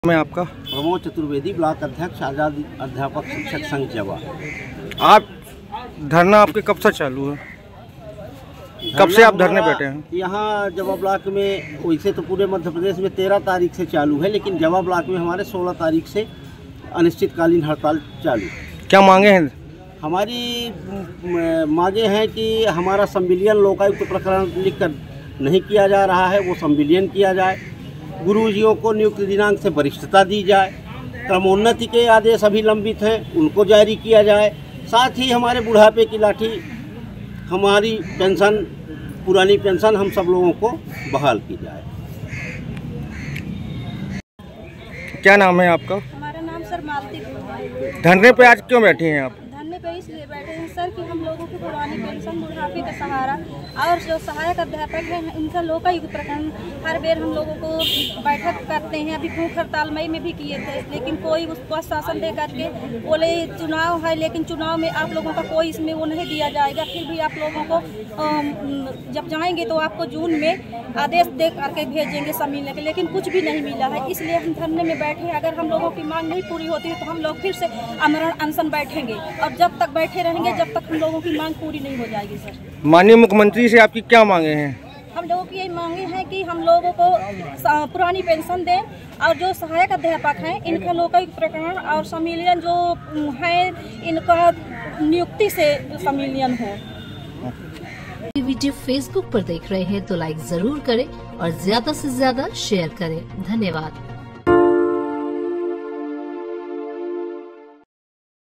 पेंशन समेत कई मांगों को लेकर अध्यापक संघ का धरना, मांगें पूरी न होने पर दी भूख हड़ताल की चेतावनी
Also Watch ►